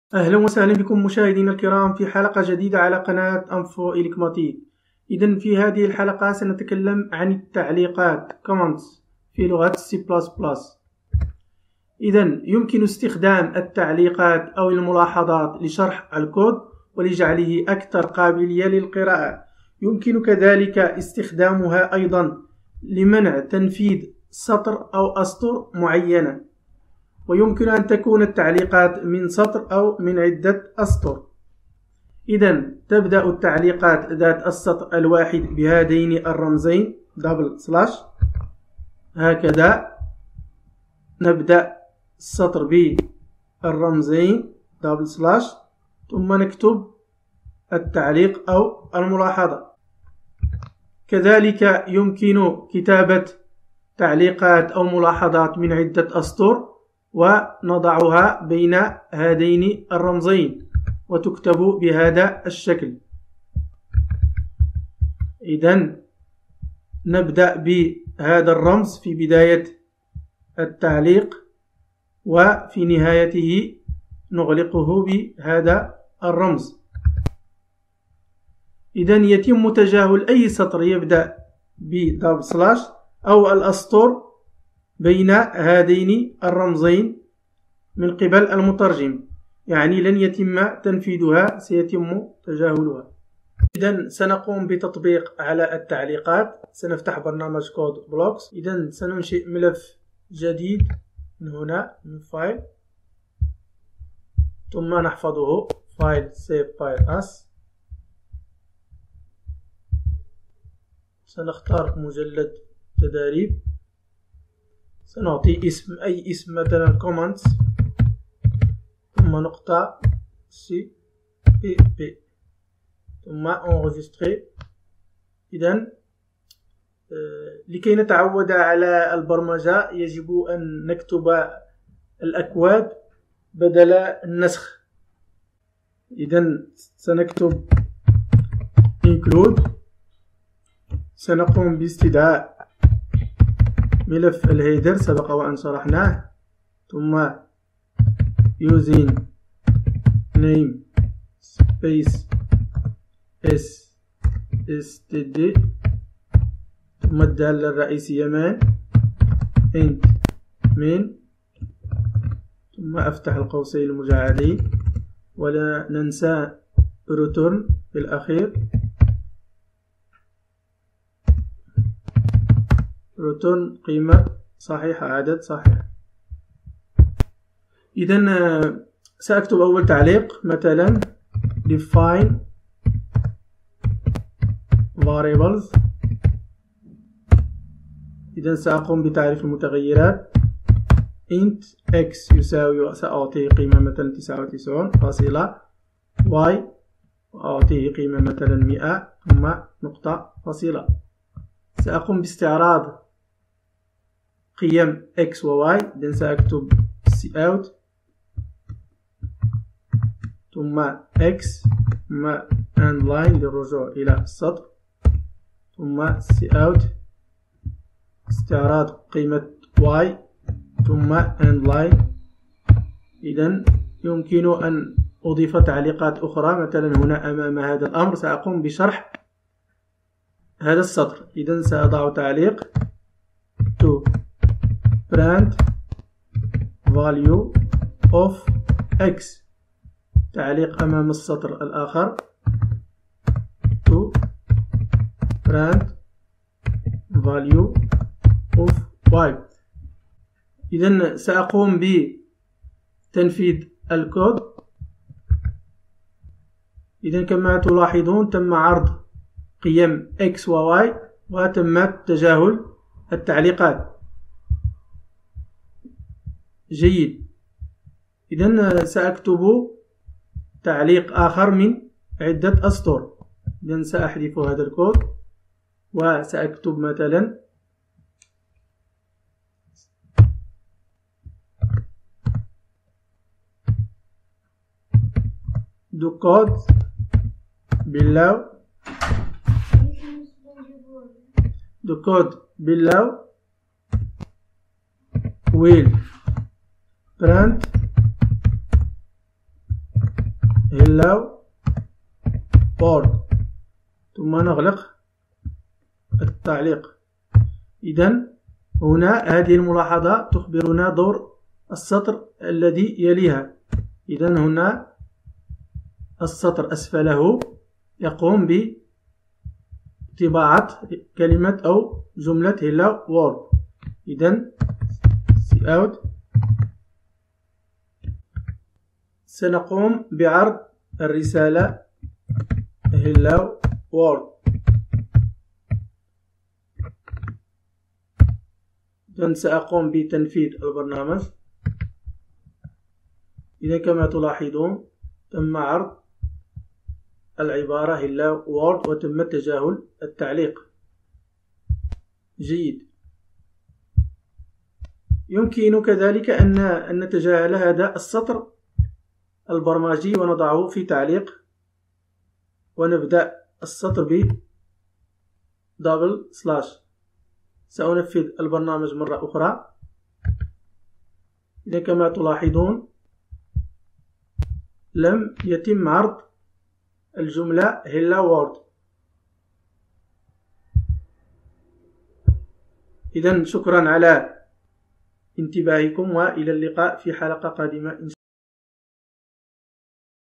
Comments in C++ التعليقات في لغة
comments التعليقات
//this is a  comment
/* comment line1
comment line2
comment line3 */
HICHAM DIOUANE